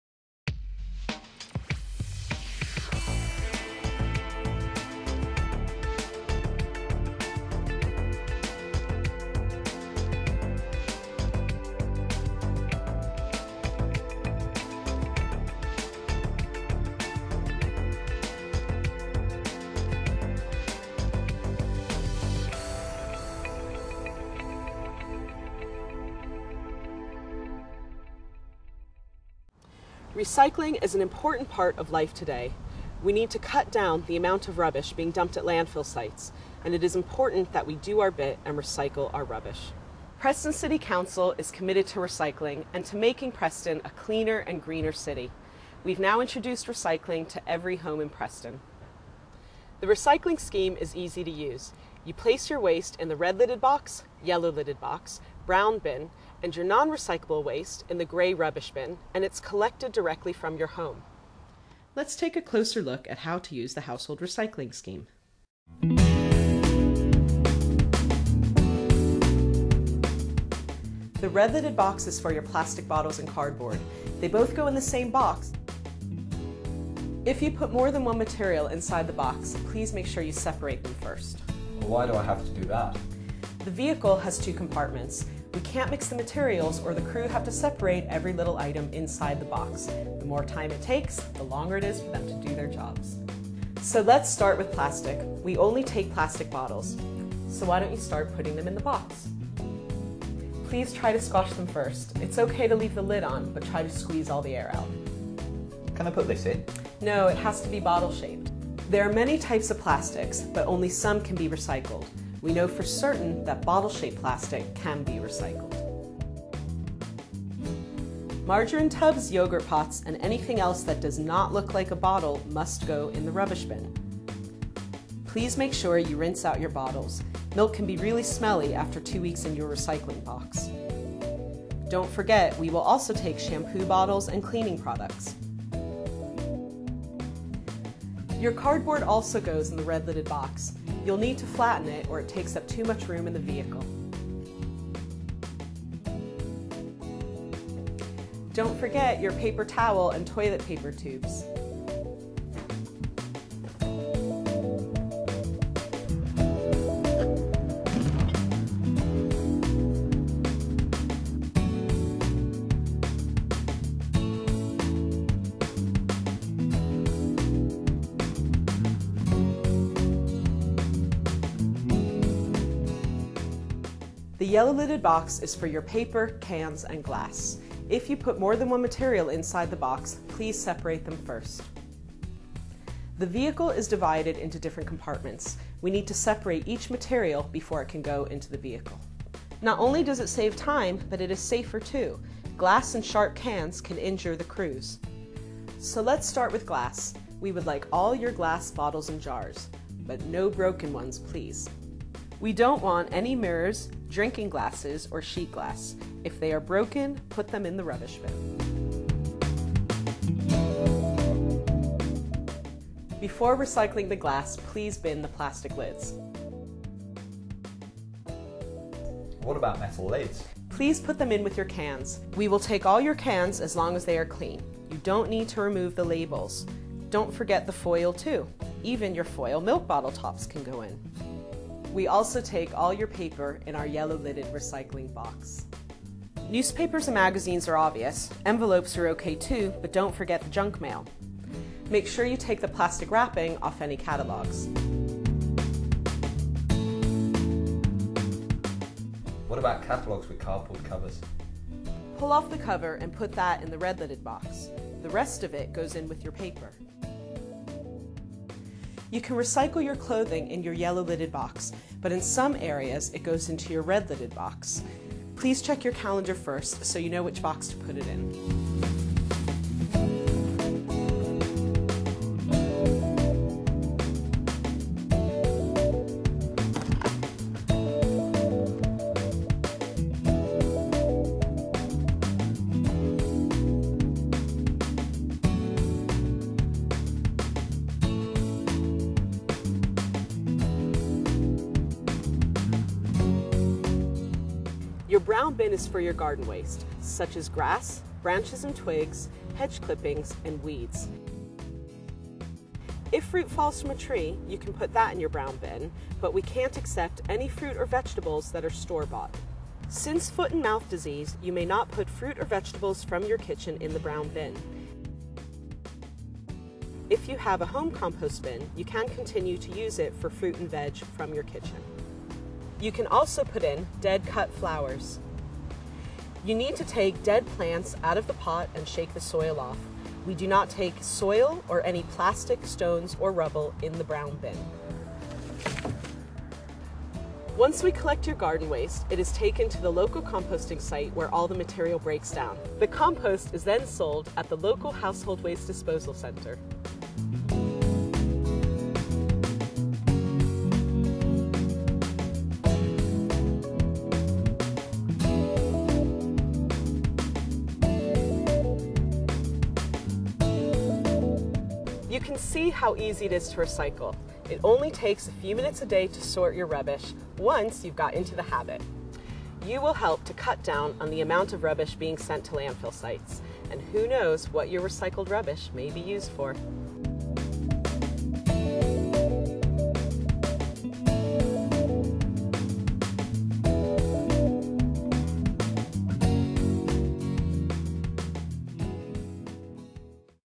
How to Throw Away Waste
Preston Council's guide to how to chuck your rubbish